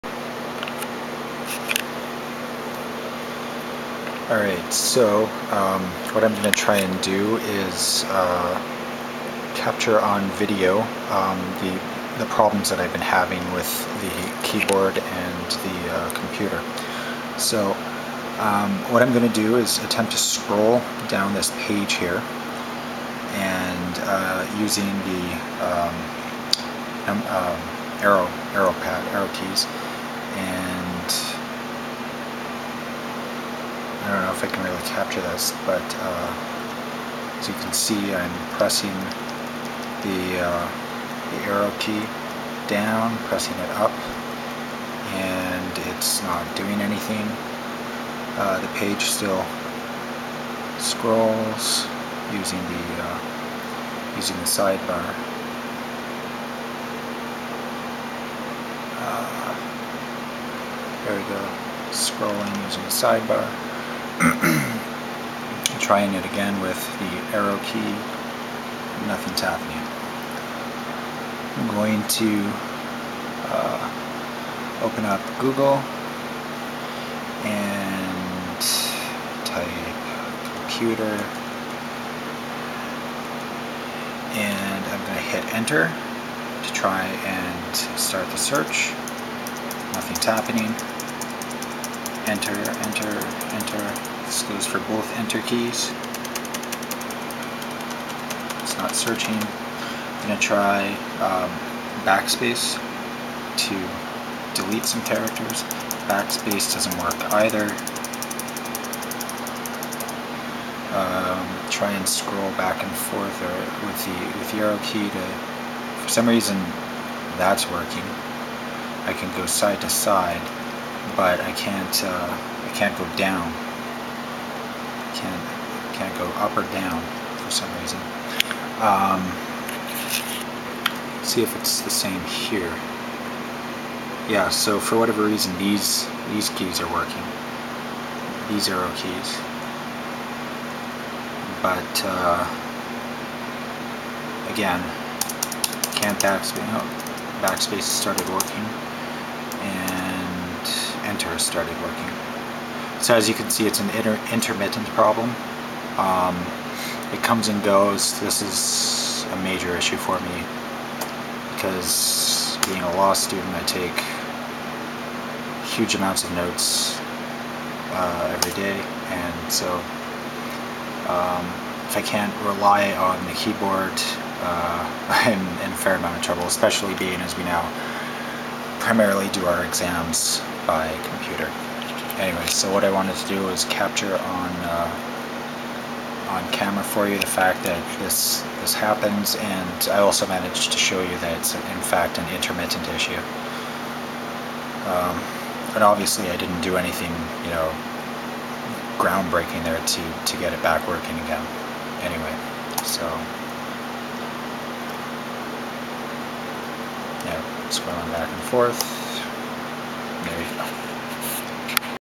video of keyboard not working
This is a video depicting the intermittent problem I have been having with my HP 4510s keyboard--spacebar, backspace, enter, and up/down arrow keys occasionally stop working.  Most recently this went on for over an hour during an important lecture while I was taking notes.

*edit (21 August 2013):  I uploaded this video just to document the issue for my local service shop, and didn't really intend for anyone else to view it.  The laptop was still under warranty, and they replaced the keyboard--no explanation was given as to what the problem was, but replacing the keyboard fixed it.